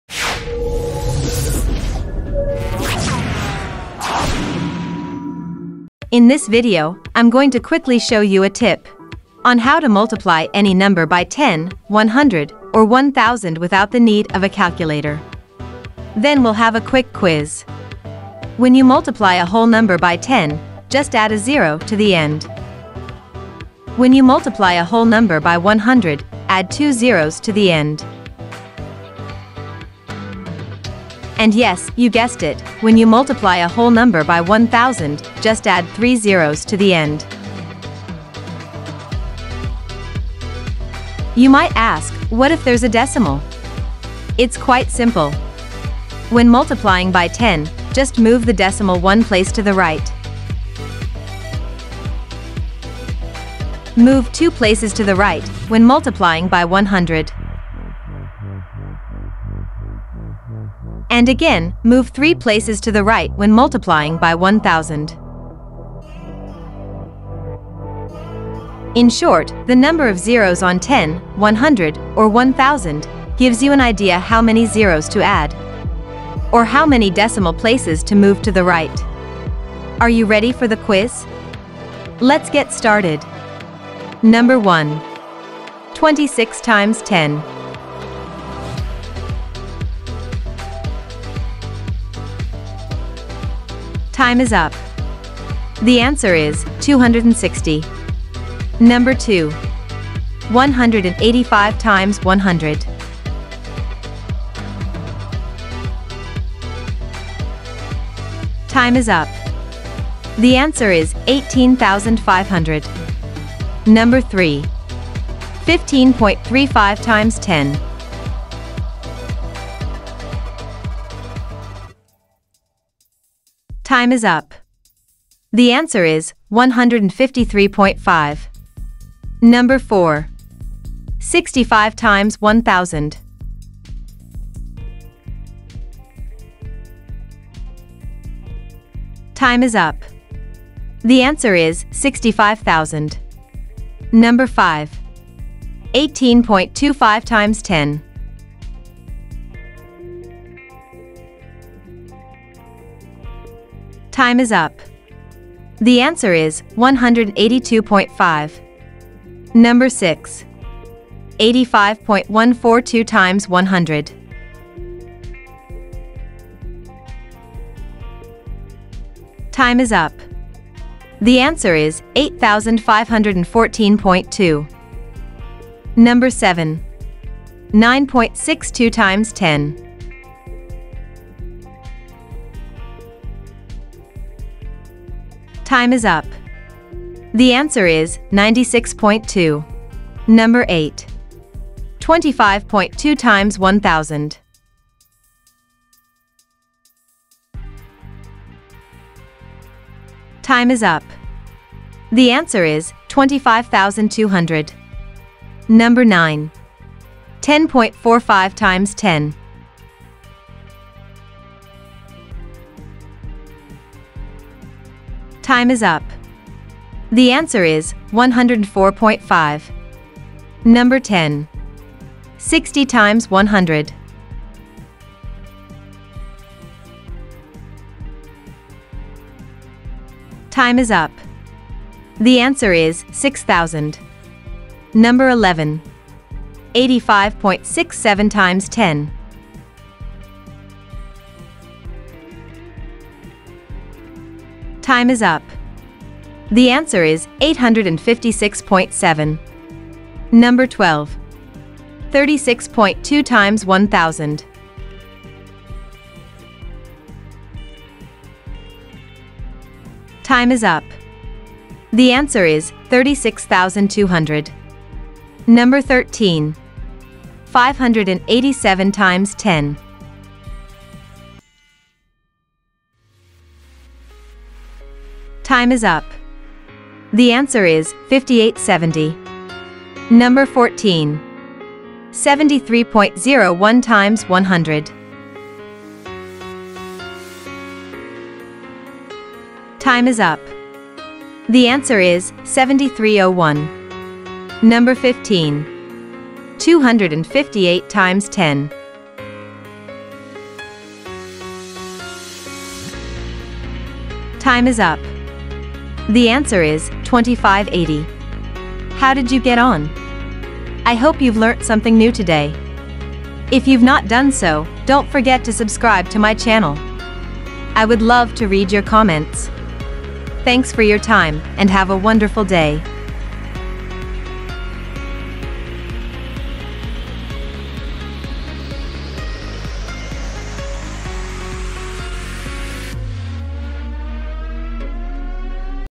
Multiplying By 10, 100, 1000 - Easy Tips and Quiz, Whole Numbers, Decimals
Maths, Math, Mathematics

Simple tips in multiplying whole numbers and decimals by 10, 100 or 1000.
Followed by a quiz.

Multiply by 10
Multiply by 100
Multiply by 1000
multiplication
whole numbers
decimals